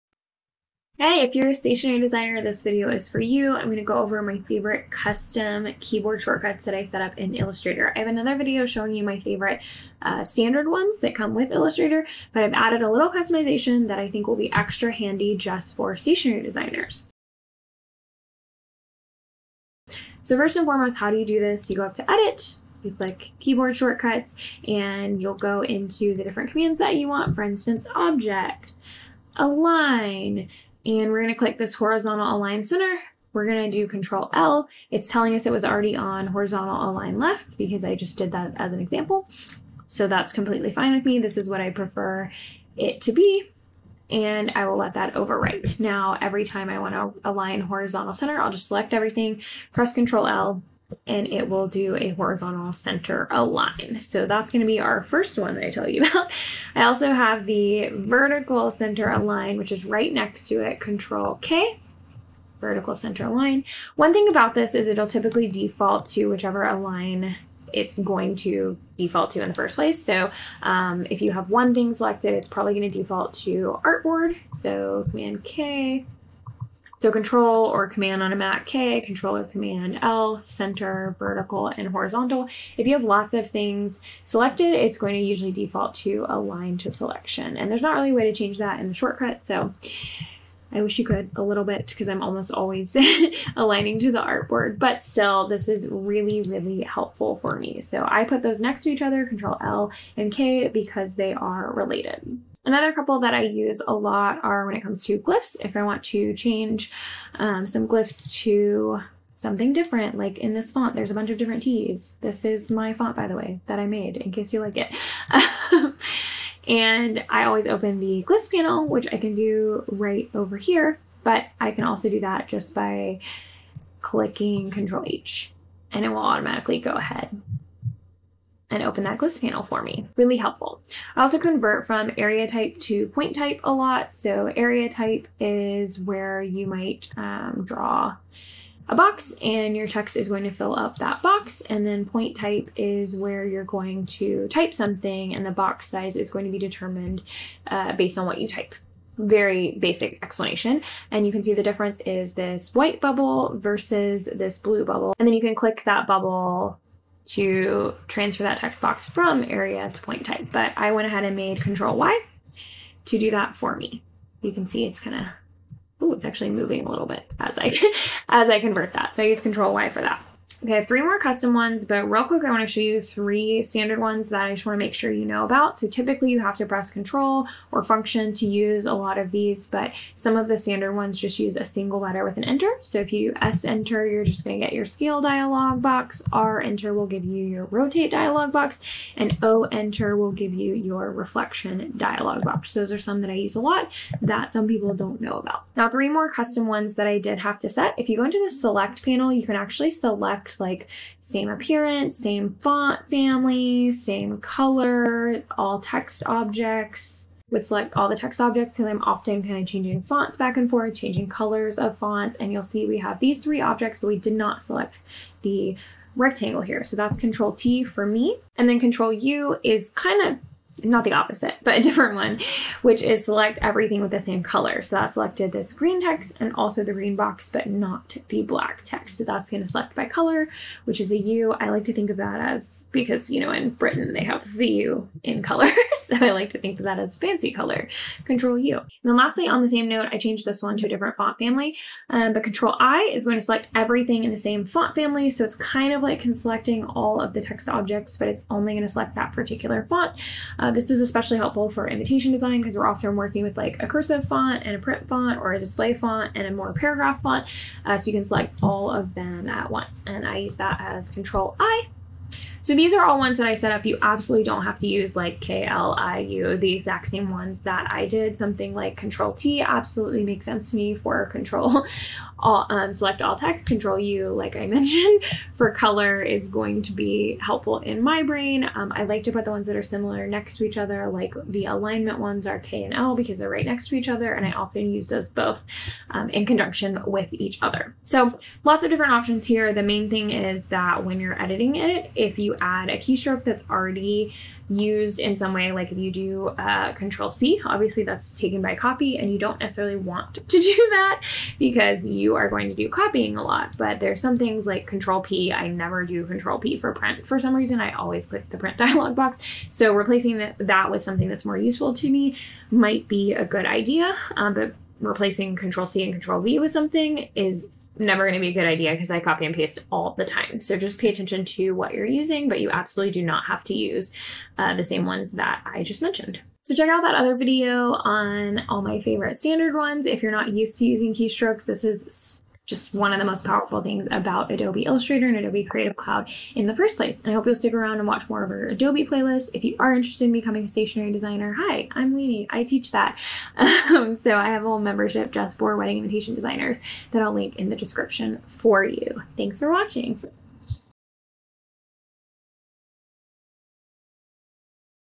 10 Custom Keystrokes in Adobe Illustrator for Stationery Designers
10 Custom Keystrokes in Adobe Illustrator for Stationery Designers

I spend a lot of time designing wedding invitation suites in Adobe Illustrator in my stationery design business. In this video I tell you some of my favorite keyboard shortcuts that I have custom made to make my process easier and more efficient!

I have another video on my favorite keystrokes that come standard with Adobe Illustrator that you can watch